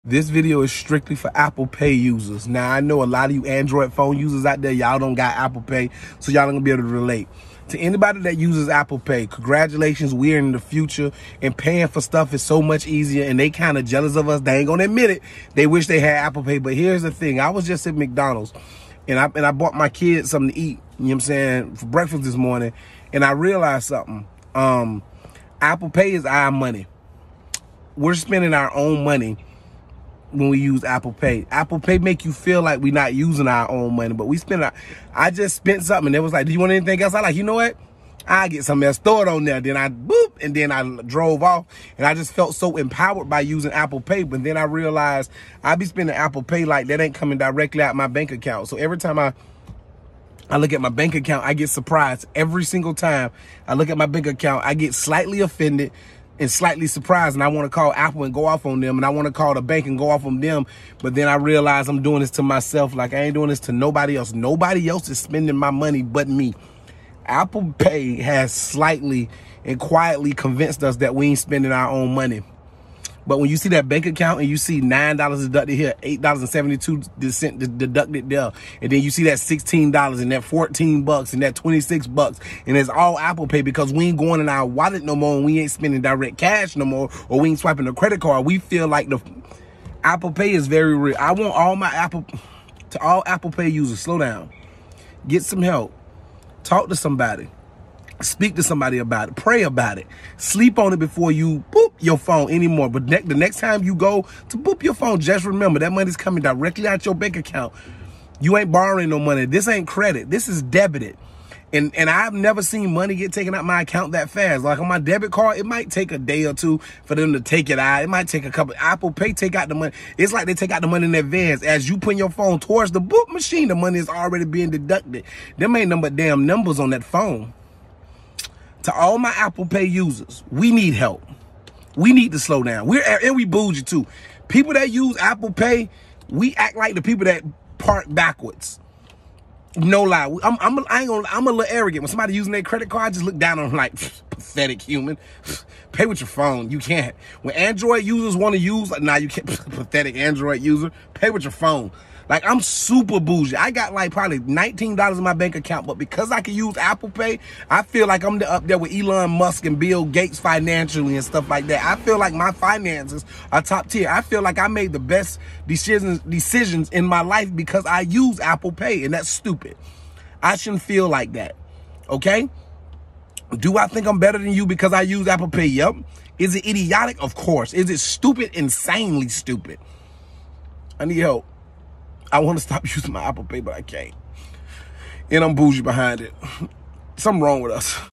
Apple Pay is not free money
Comedian Josh Pray urges Apple Pay users to pay slower.